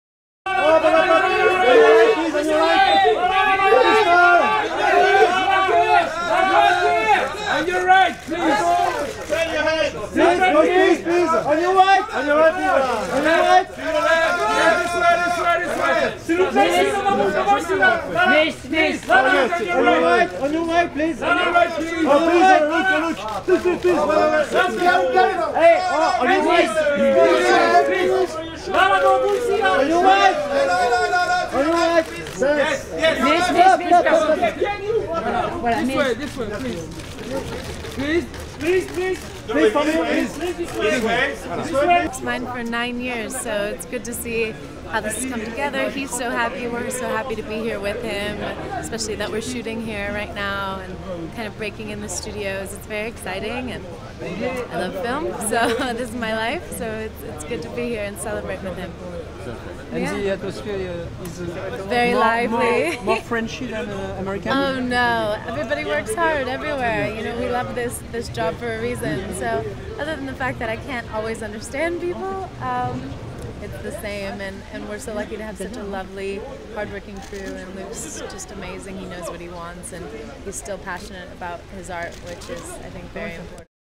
Dianna Agron at the Cité du Cinema opening 2012
Dianna Agron at the Cité du Cinema opening in Paris, France. September 21, 2012.